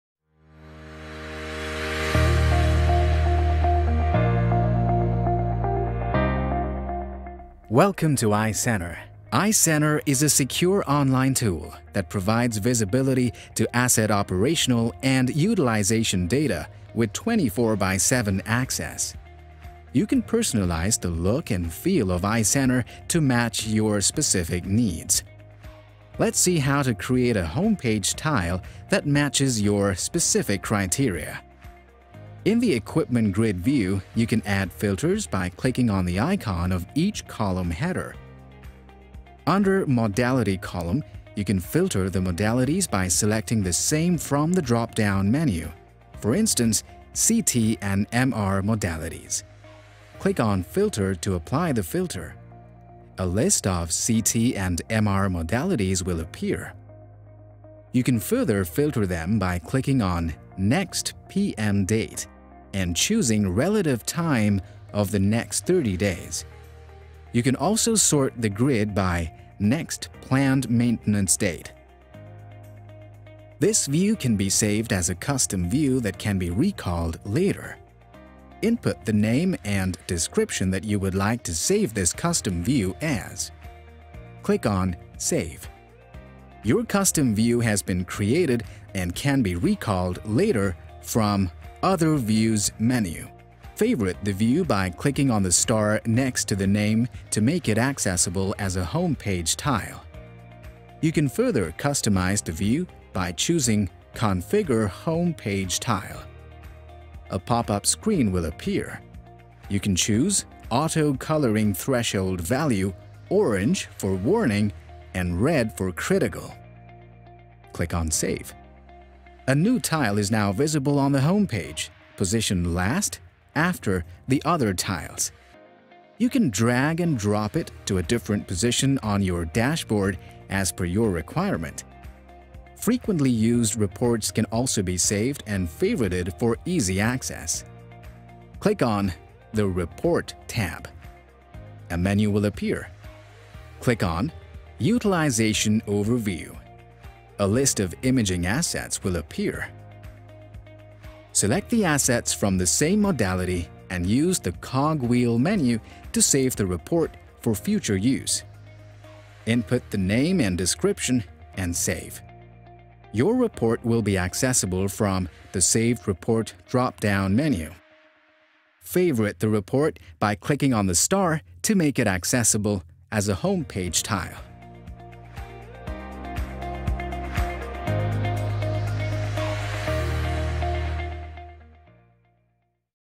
Learn how to customize GE Healthcare’s iCenter for your facility
In this short video, you’ll see how easy it is to personalize your iCenter account to monitor Equipment Service Request and Utilization.  GE Healthcare’s iCenter is a secure (cloud-based) online tool that provides 24x7 visibility to asset operational and utilization data.